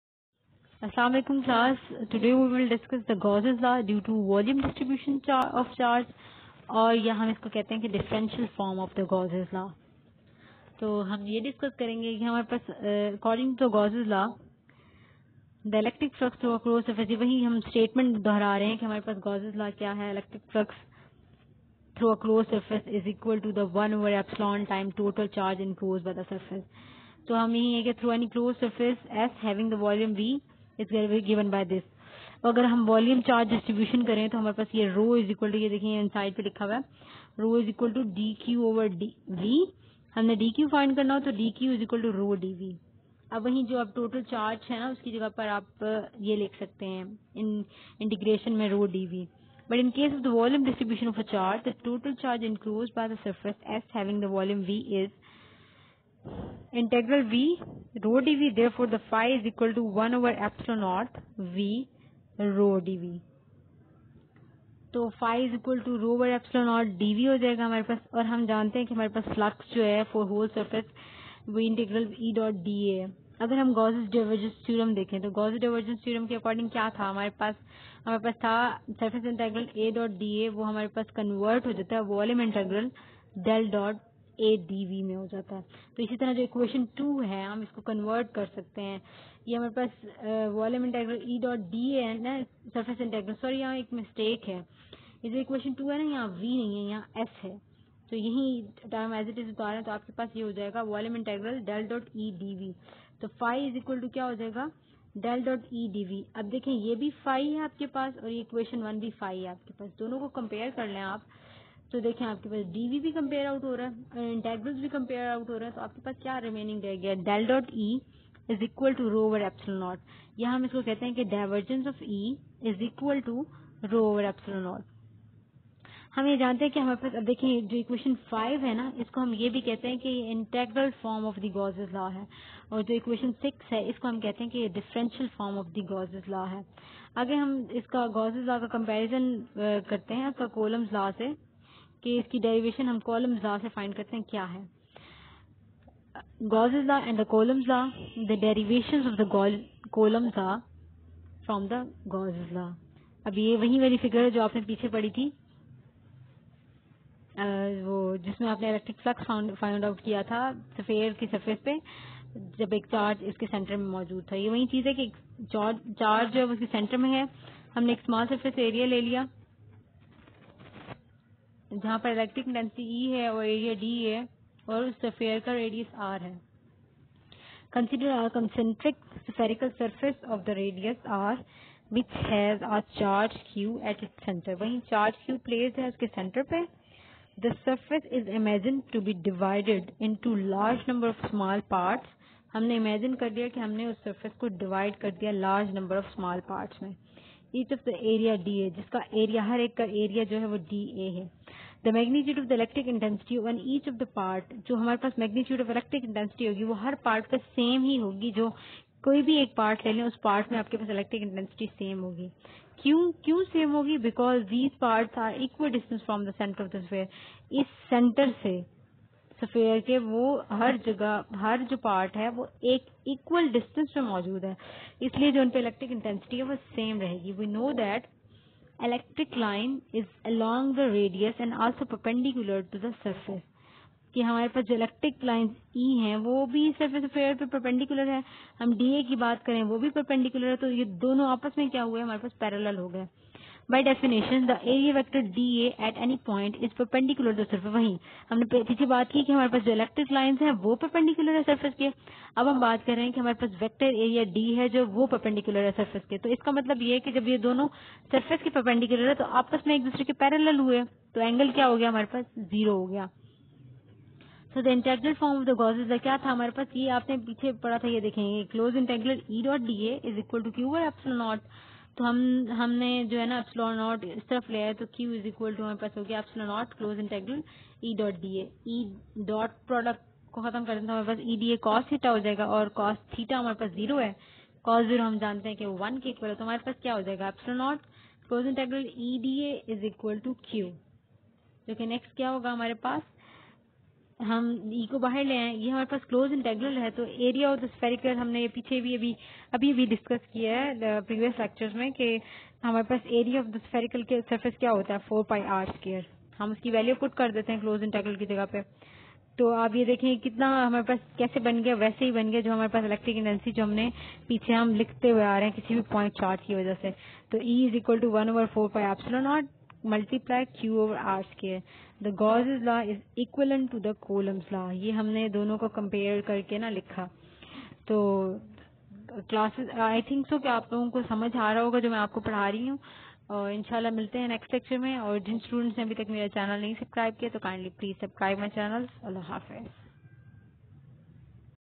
Electricity and magnetism#Lecture no.6#Differential and integral form of Gauss's law#comparison.
Gauss's Law due to volume distribution of charge or differential form of the Gauss's law, Gauss's law and Coulombs law, Derivation of coulomb law from Gauss's law.